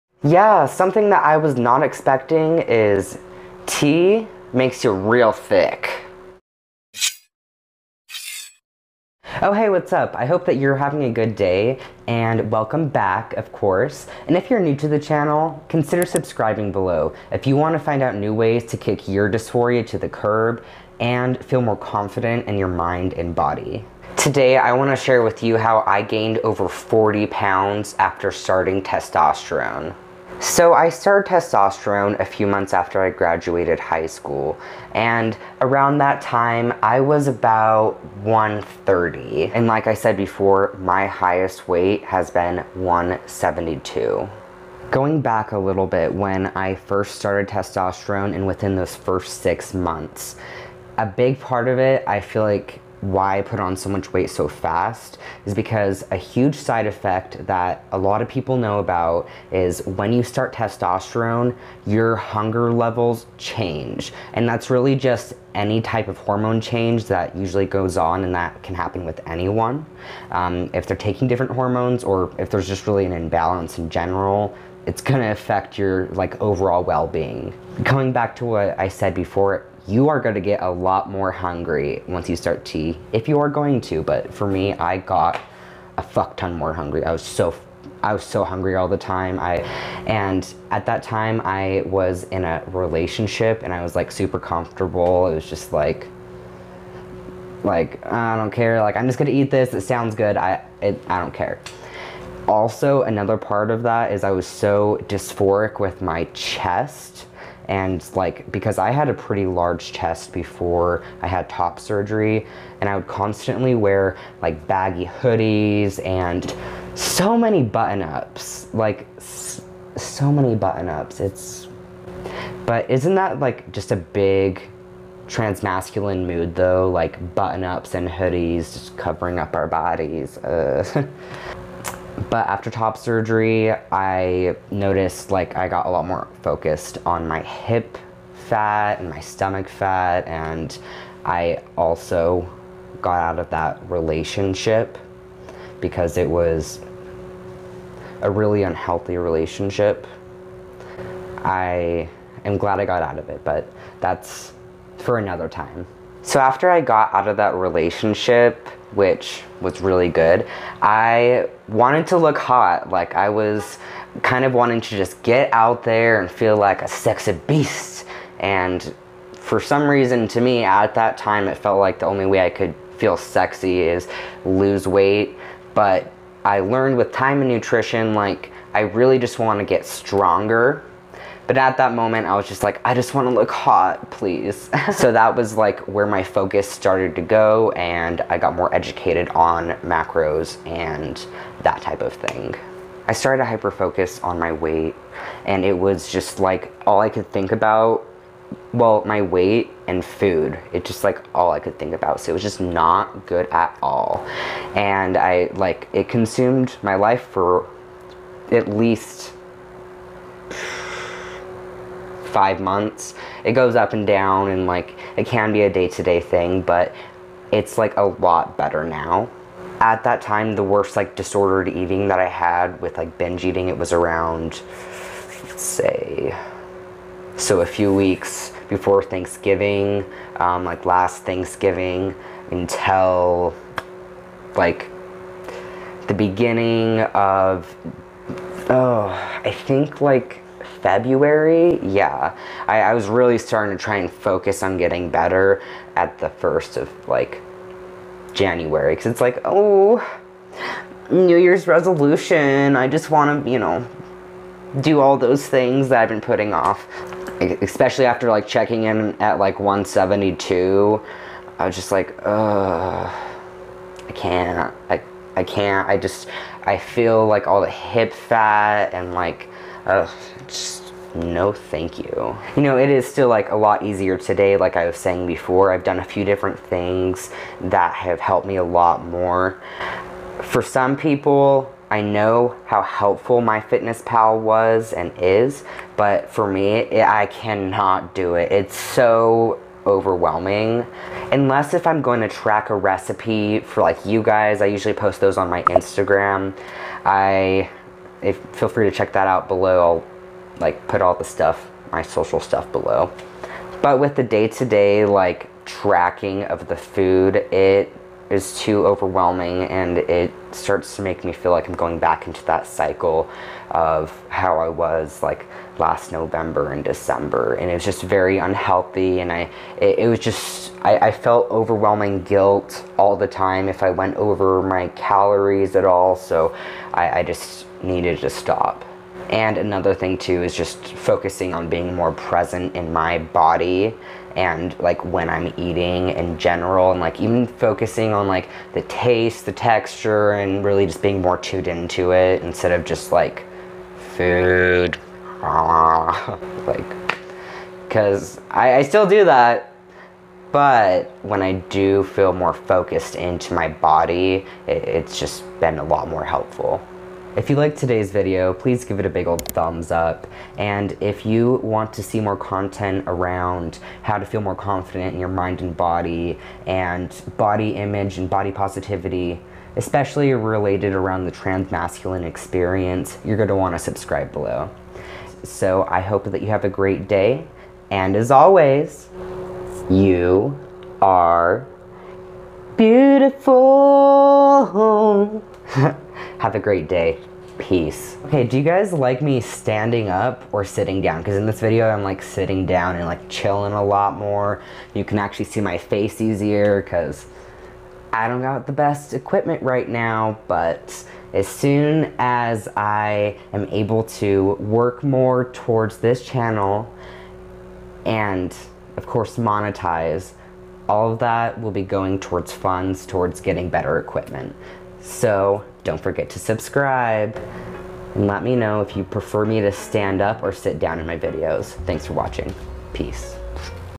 Why Testosterone Made Me Gain Weight
In this video I share with my experience with gaining over 40lbs on testosterone. Within the first 6 months on being T I gained the first 30 pounds. I was comfortable in a relationship and had a lot of chest dysphoria so I was focused on that.

After getting out of my last relationship and having top surgery I wanted to focus on getting in better shape. I started to hyper focus on my body and weight and ended up gaining 12 more pounds instead of losing weight. I was binge eating every day.

With a few tips and tricks I have been able to break my binge eating cycle for the most part.